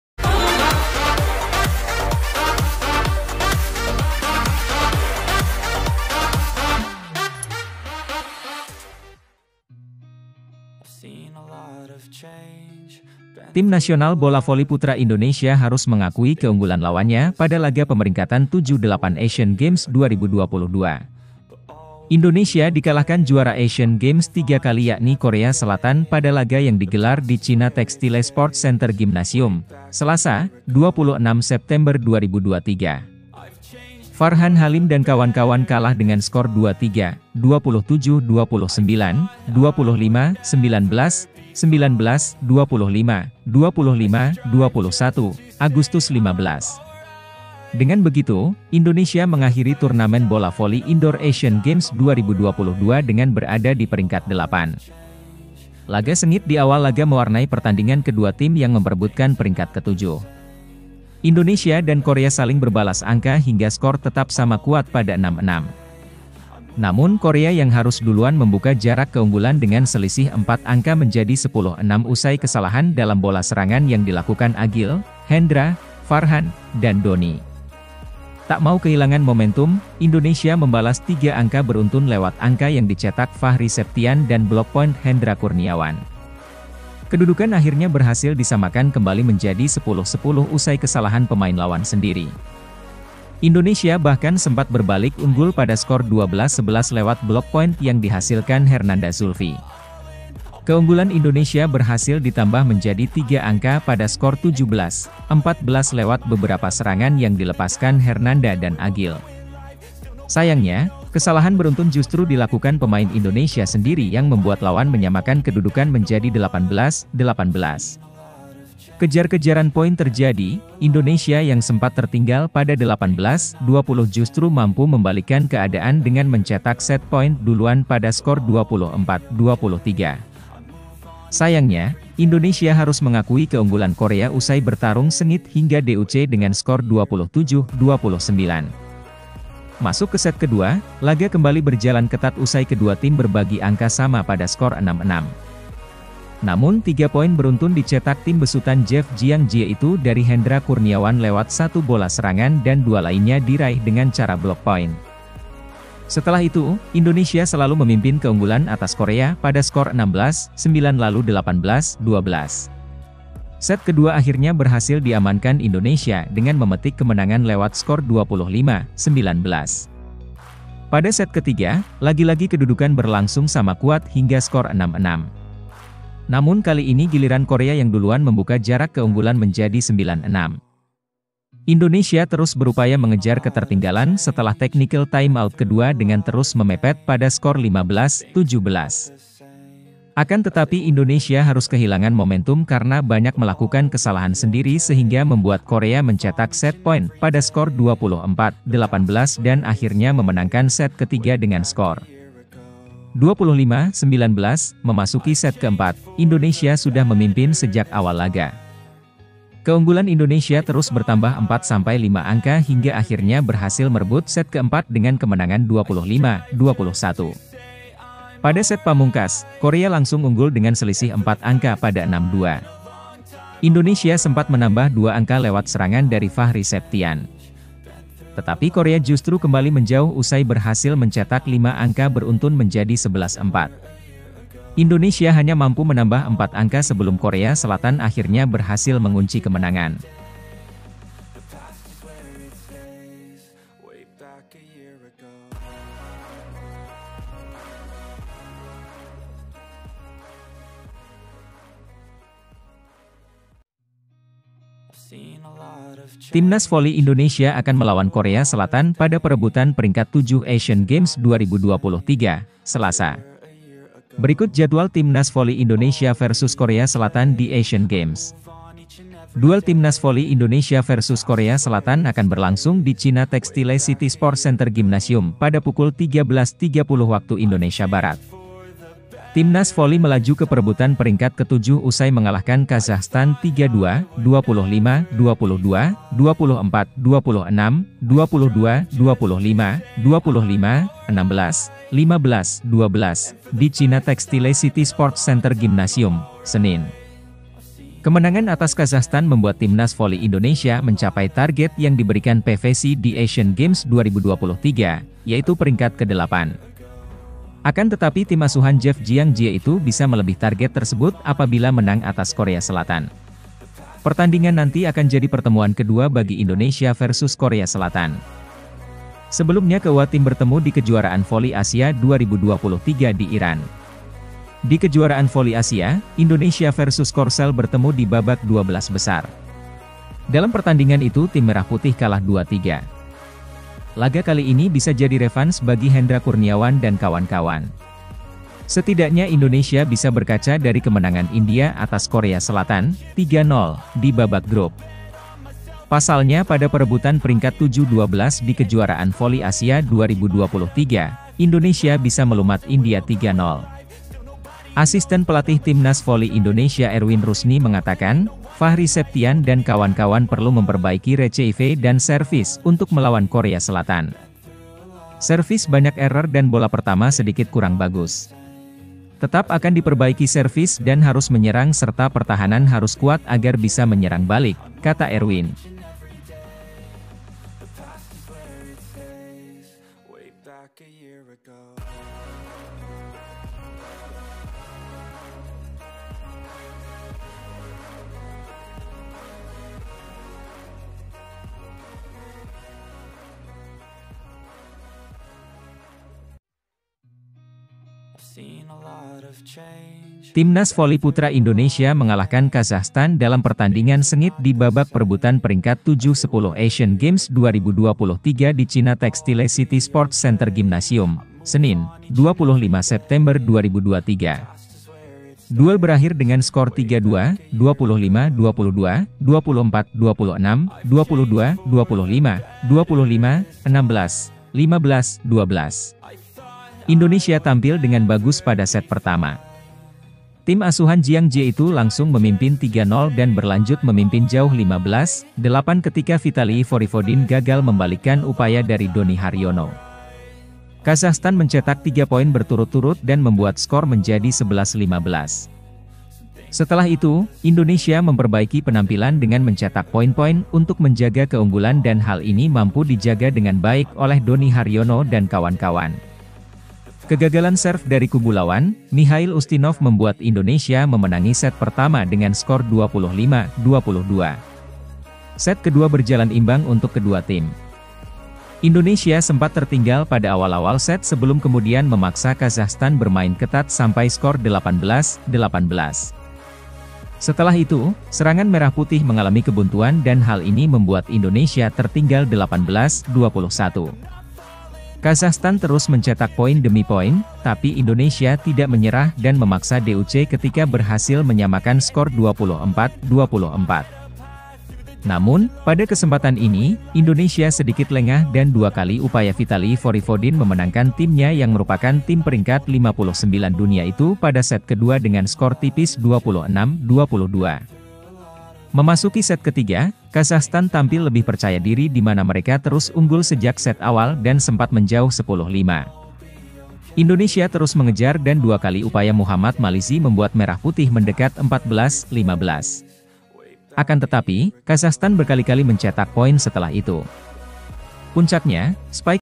DIGEBUK KOREA SELATAN❗JEFF JIANG GAGAL PENUHI TARGET PBVSI DI ASIAN GAMES CHINA, CUMA RANKING 8
SUMBER BERITA